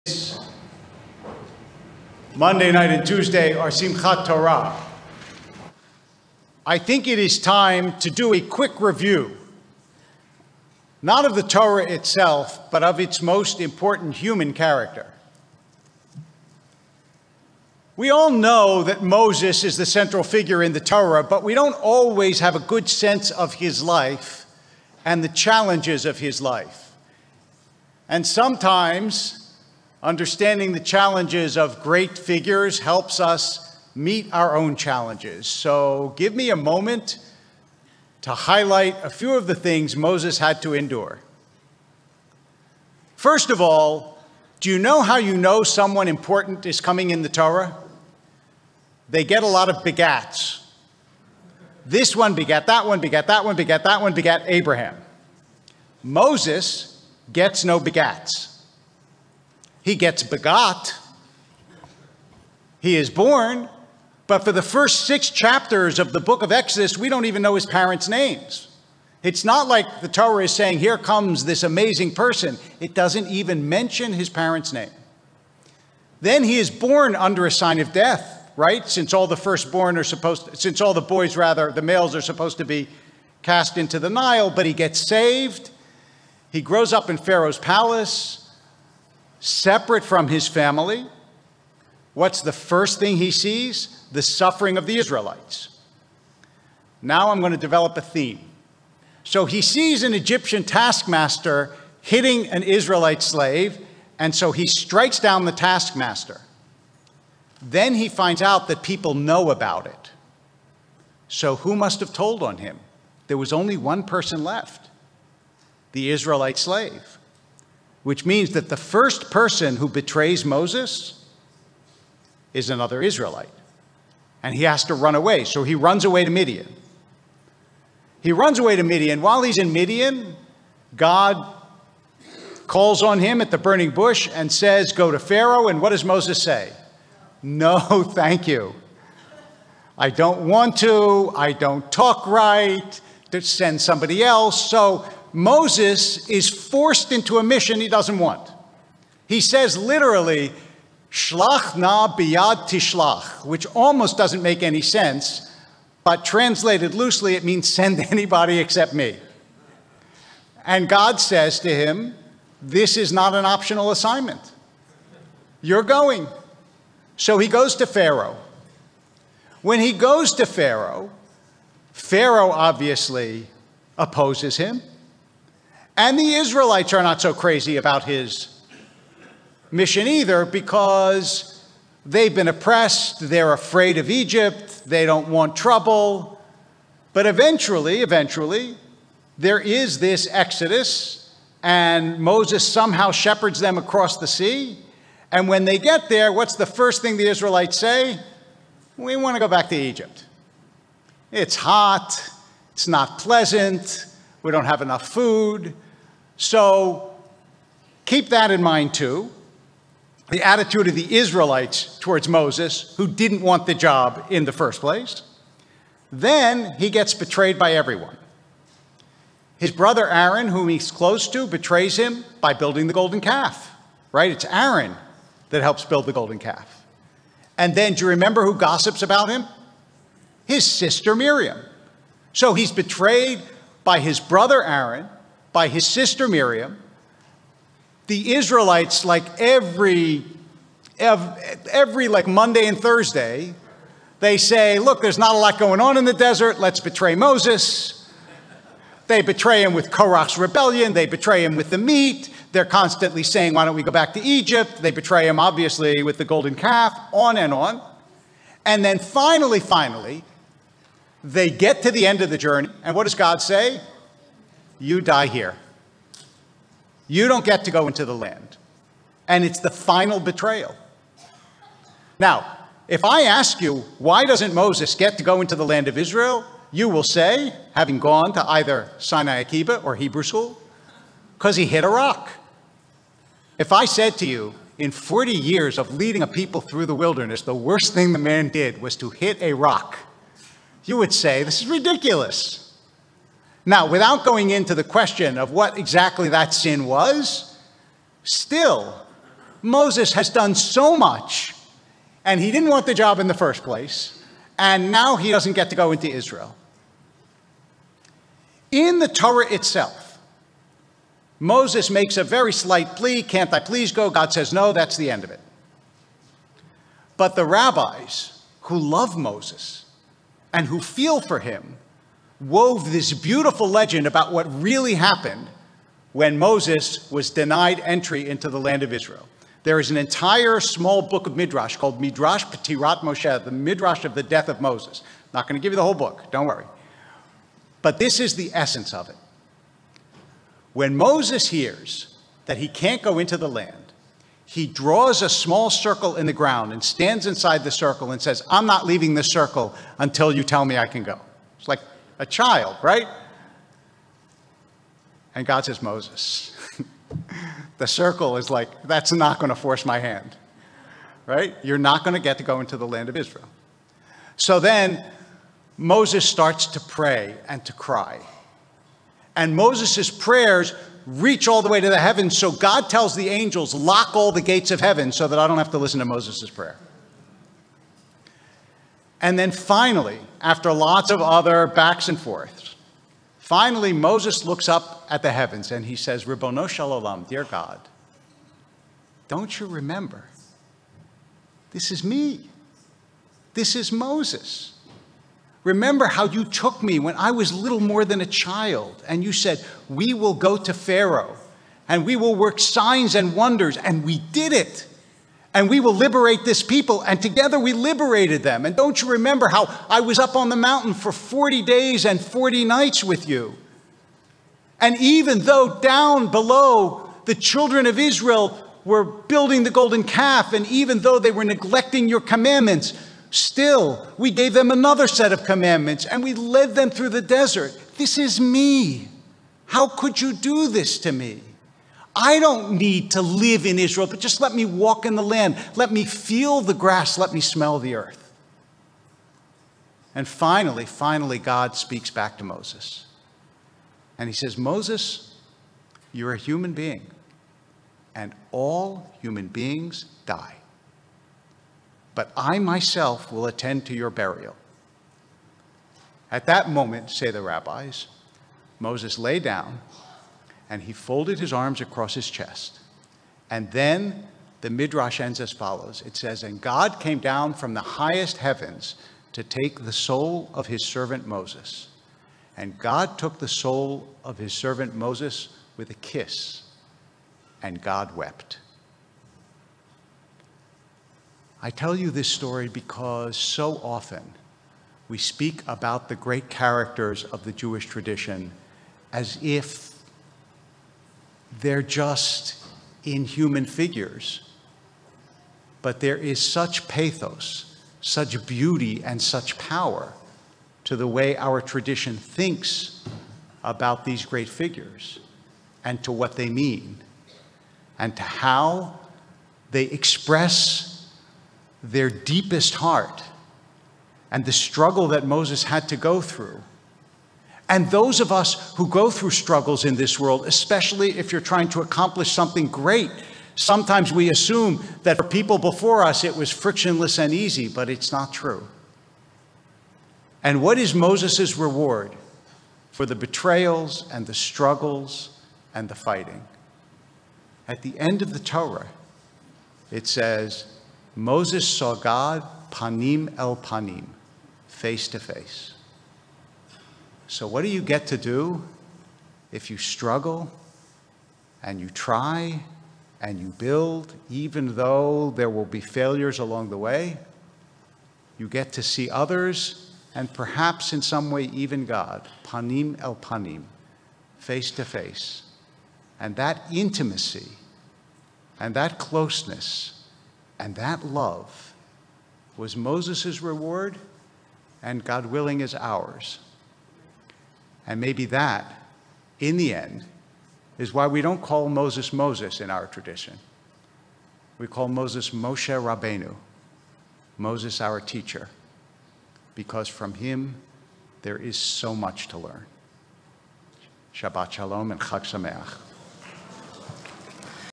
"The Life and Death of Moshe Rabenu" | Sermon by Rabbi David Wolpe
Ziegler Sanctuary, October 15th, 2022.